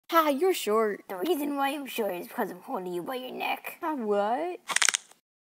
You’re short
I made another Megaman animation cuz a few of you guys wanted me to make more. OF COURSE I’LL MAKE MORE, And my summer just started so I have all the time to make more. But I also have other plans so be patient! Anyways enjoy like 5 seconds of content lol. I’ll make more when I think more.
Oh yeah and the context of this video. This is a little headcannon that Bass and Quickman are brothers. They were both built by Wily so that technically means they’re brothers. They also live together in the same place at Wily’s castle. This is just some typical brother shenanigans they get into just like Cutman and his bros.